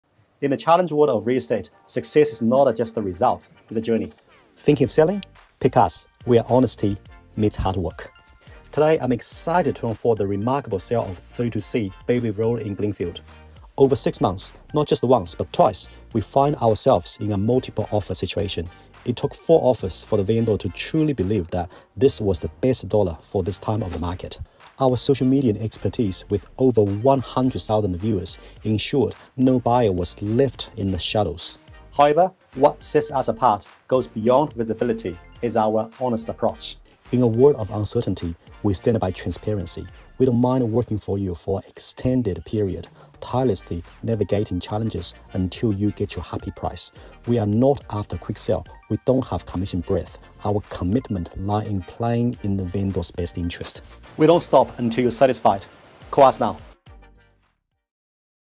🏡 SOLD: 32C Bayview Road, Glenfield! 🎉
Navigating market complexities over six months, we achieved a remarkable sale at 32C Bayview Road in Glenfield. Twice in multiple offer situations, it took four offers for the vendor to embrace the best deal for this market.

Looking to buy or sell in North Shore? Trust us to navigate every market twist.🌟